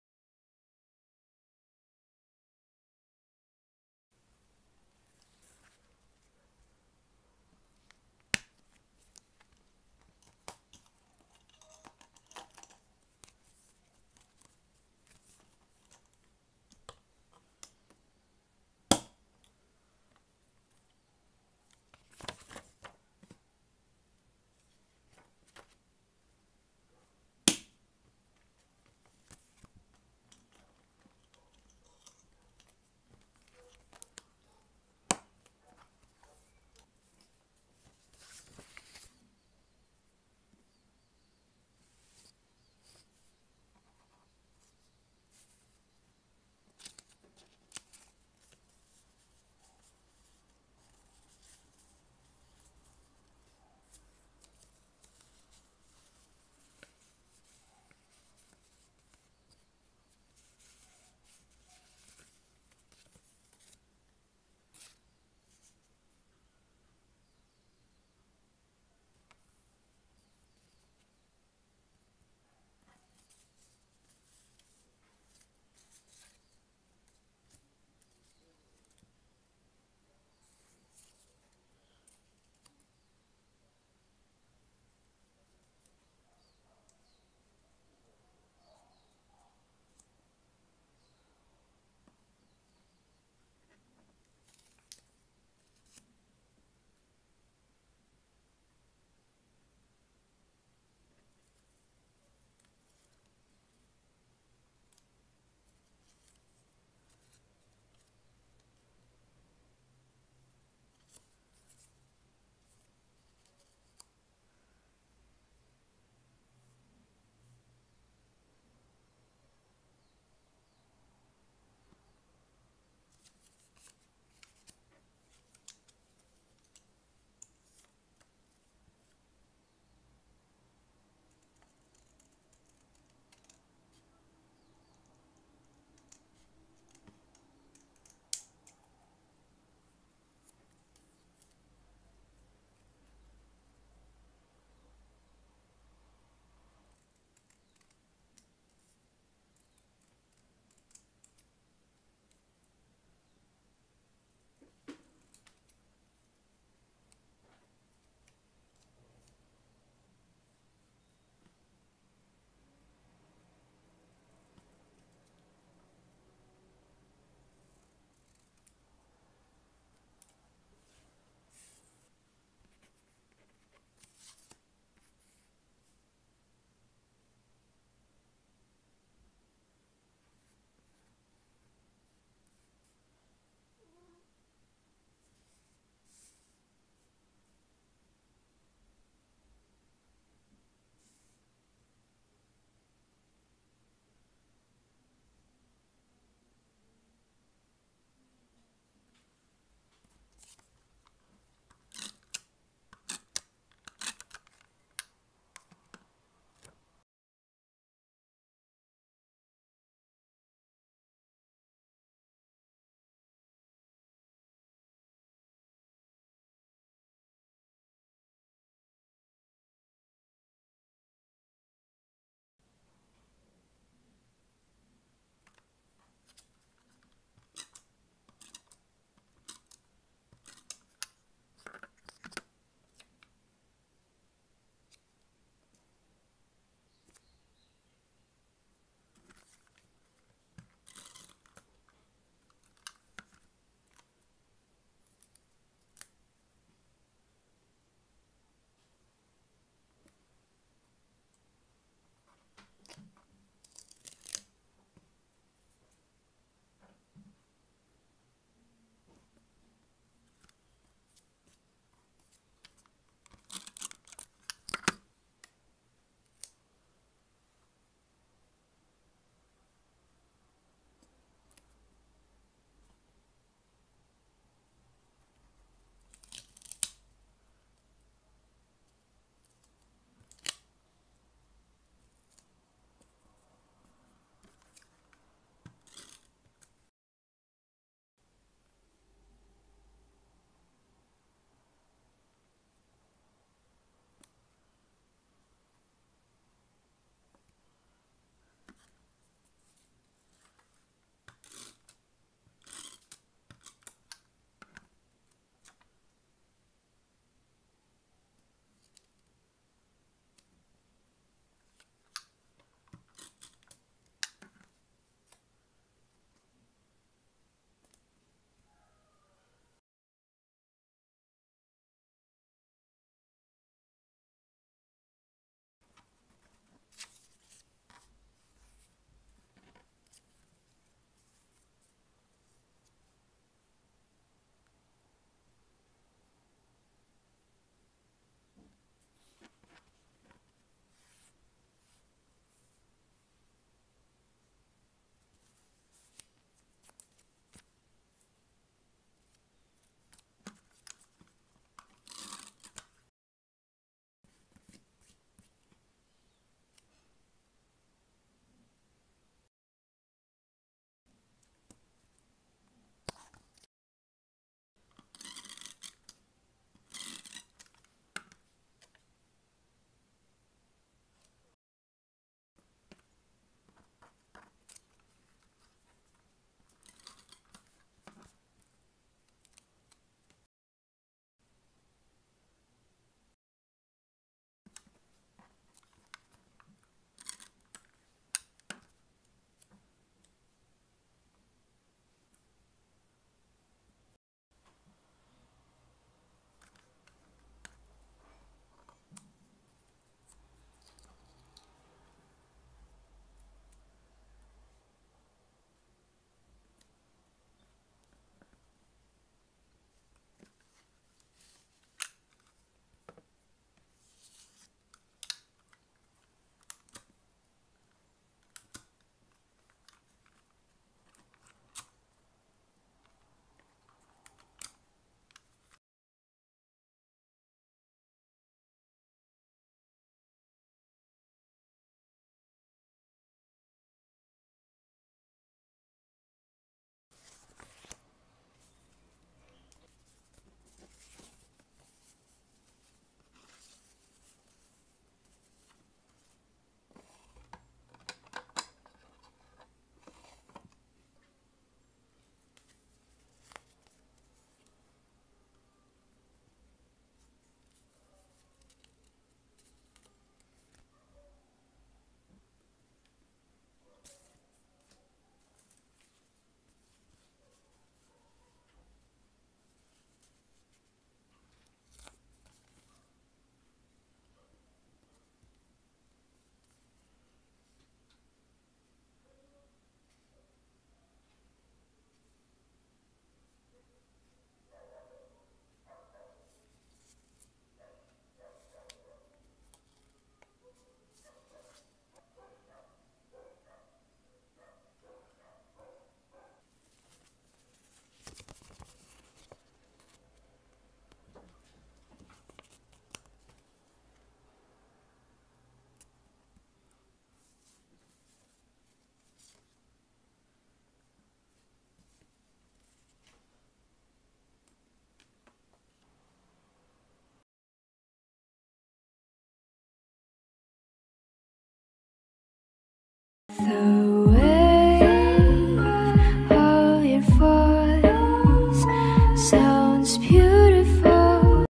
asmr anime journal with me: inuyasha! | journalingwithye
oioi migus,
o vídeo de hoje é mais um anime journal with me, mas dessa vez numa tentativa de asmr HDUASHDUASHD
fiz um spread sobre meu anime favorito da vida, e o primeiro hihi, inuyasha! me conta aqui em baixo se você conhece.
enfim, espero que vocês gostem, não esqueçam de curtir o vídeo que me ajuda bastante e nos vemos no próximo 💜

➥stickers usados:
mandu
sunnysideup

🌷 trilha sonora desse vídeo:
nenhuma dessas musicas me pertence, dê suporte a esses artistas incríveis!